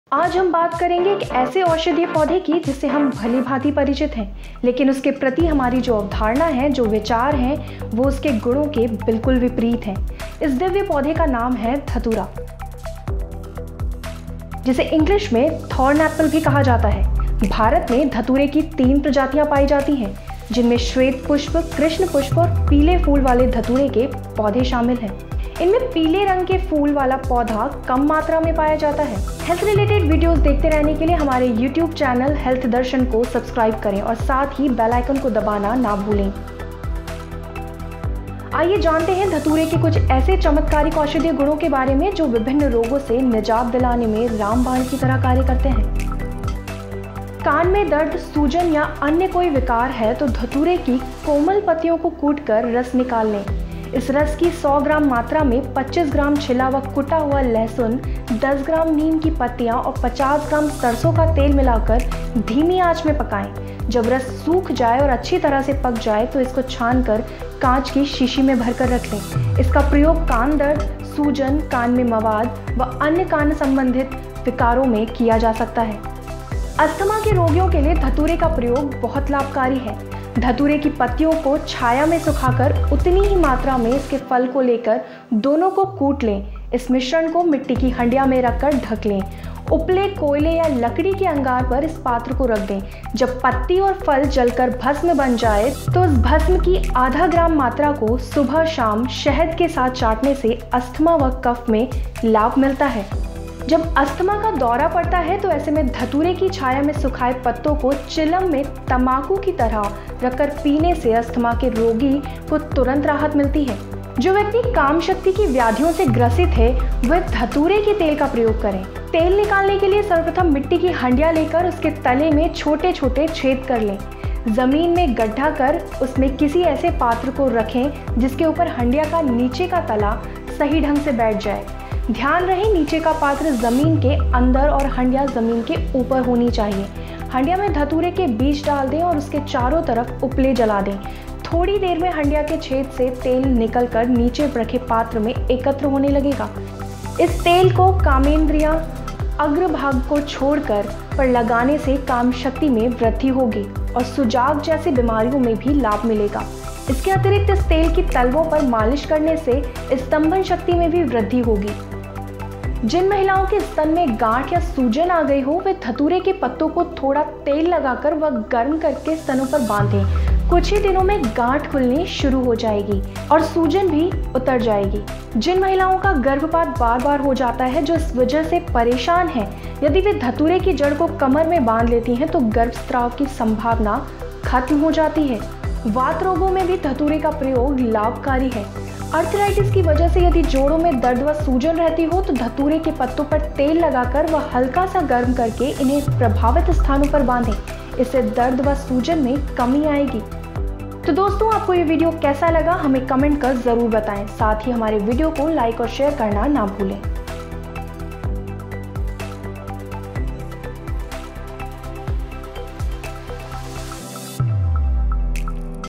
धतूरे के चौंका देने वाले फायदे I Asthma, Sexual problems, Breast Cancer जैसी बीमारियों का इलाज़
Through this video we are sharing some ‘Home Remedies’ of Healthy Life for people.  Today we are going to talk about Datura which is also known by the common name thorn apple. It’s a plant in the nightshade family. Several Datura species are collected for use as drugs,

Keywords - Ear pain, asthma, sexual problems, breast cancer